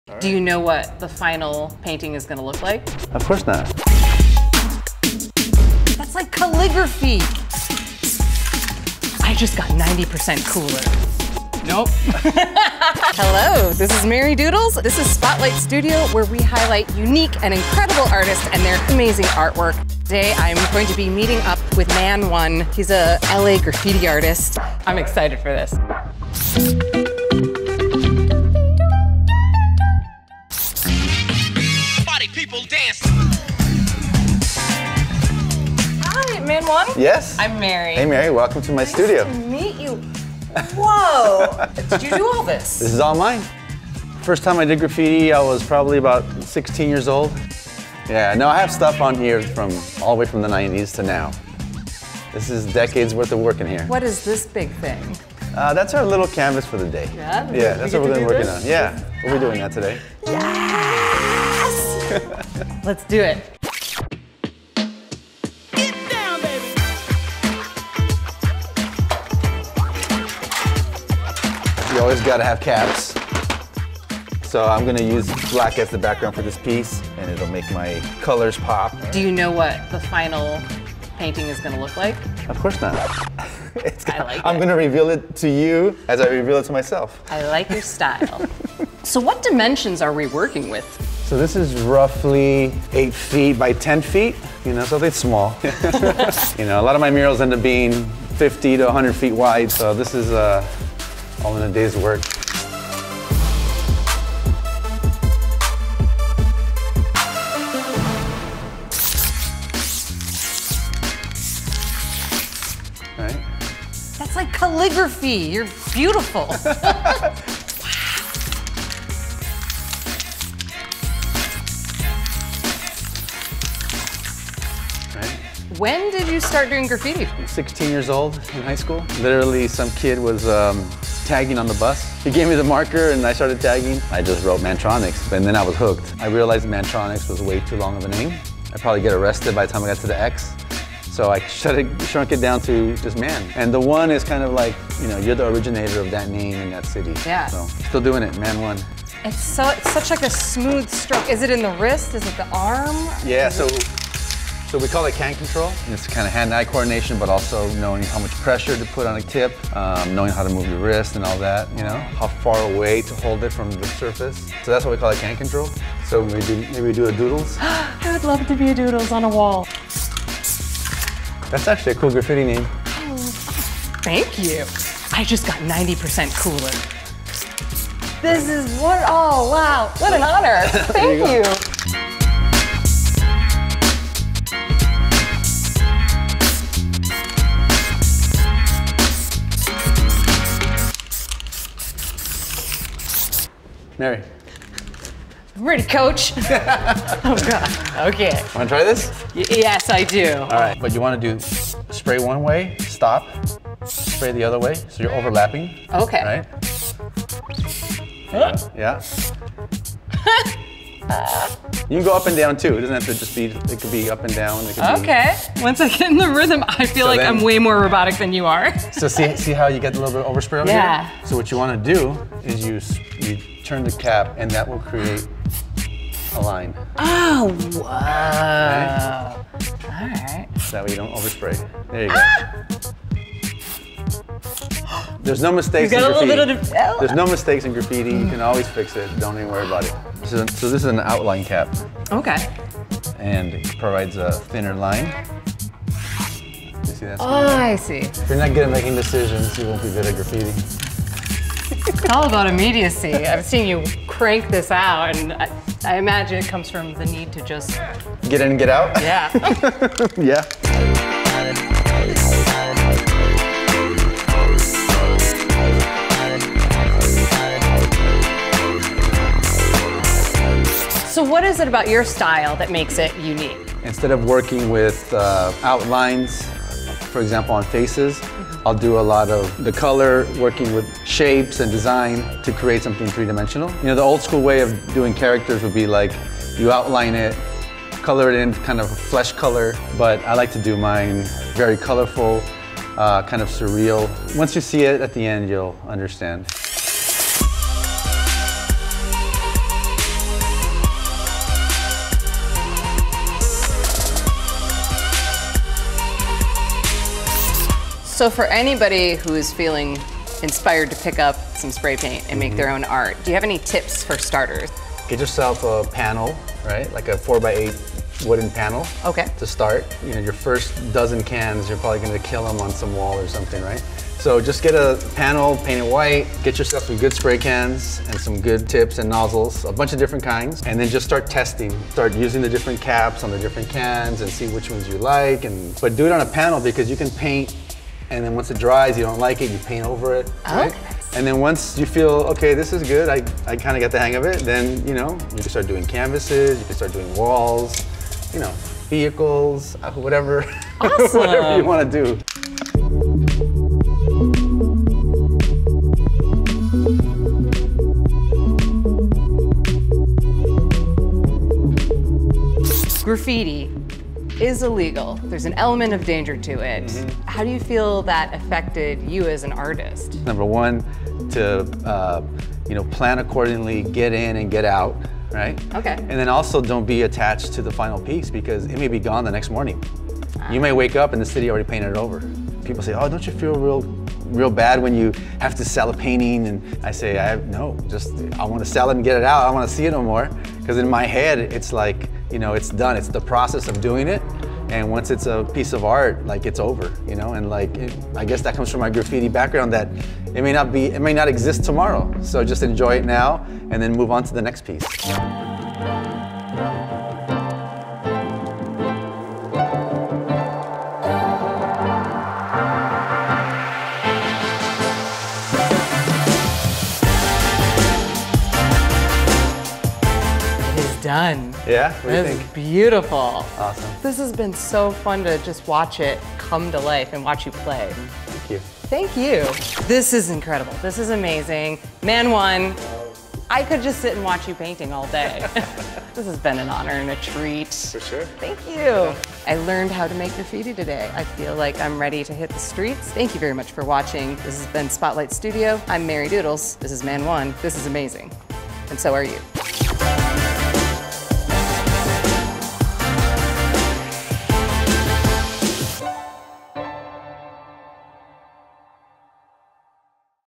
Graffiti Art with Man One | Spotlight Studio (FT. Mary Doodles)
Welcome to Spotlight Studio! Join our host Mary Doodles as we meet artists who use unusual methods and mediums to redefine the meaning of art.

In the third episode, Mary Doodles learns the art of graffiti with LA-based artist Man One!
Who should Mary Doodles invite to the Spotlight Studio next? Tell us your favorite artists in the comments!